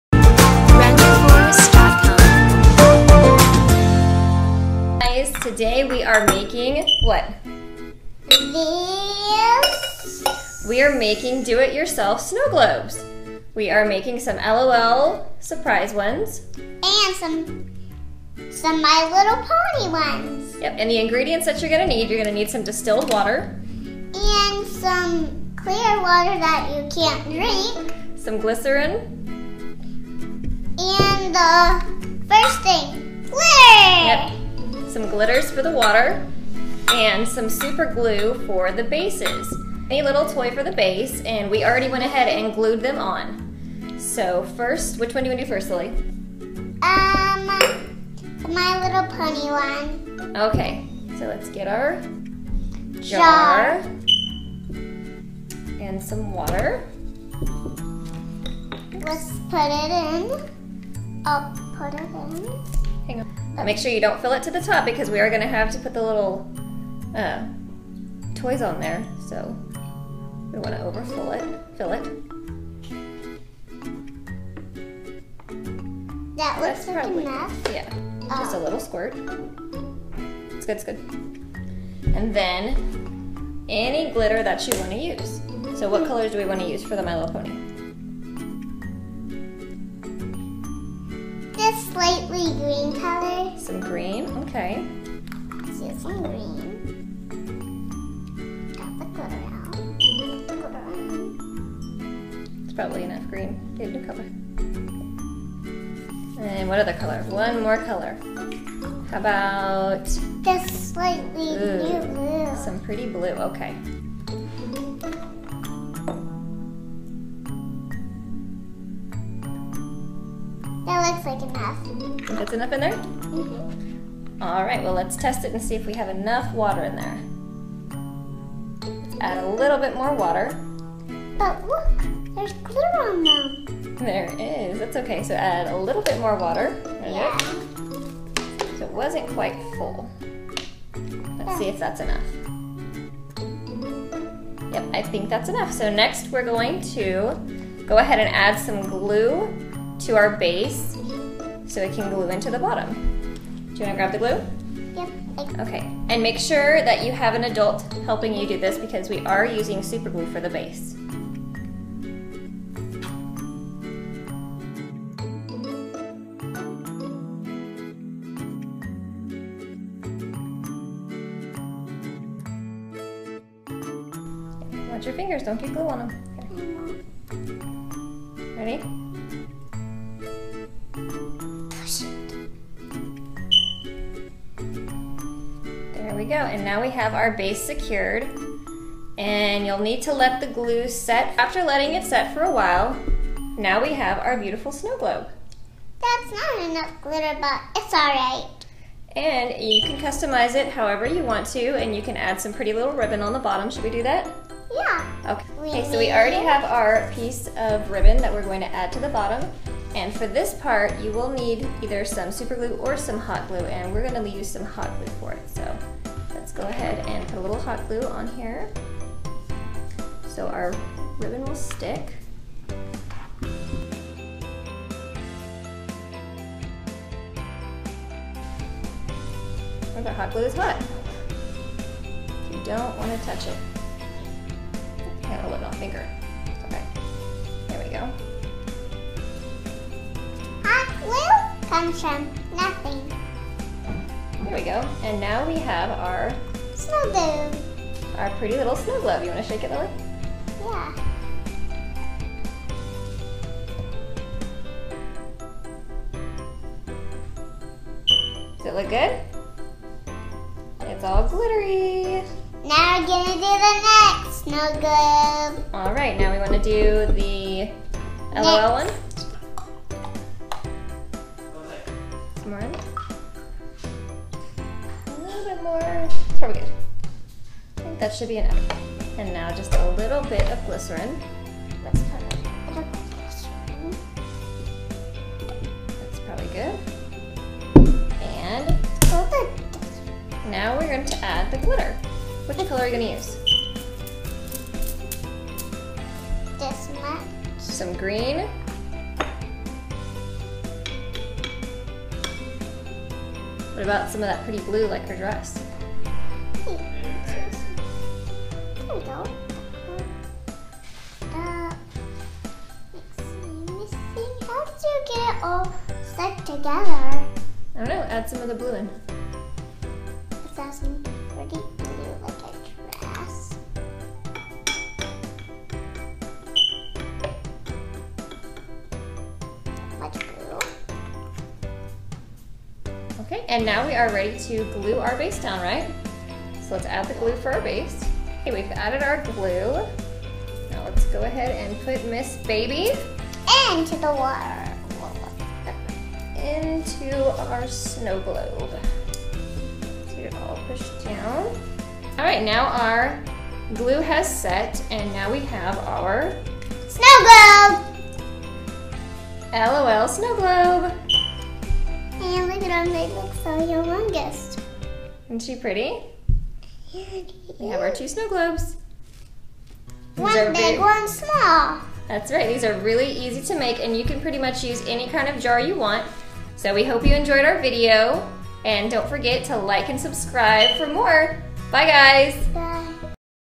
LOL Surprise DIY Glitter Snow Globe
In today's video Lily and I are making D. I.Y LOL Surprise Doll glitter snow globes with L.O.L Glitter Surprise dolls , L.O.L Little Sister dolls, and My little pony's  with Fluttershy and Diamond Tiara. This is a fun DIY project and would make a great Christmas gift for kids.

Check the links below for more fun videos!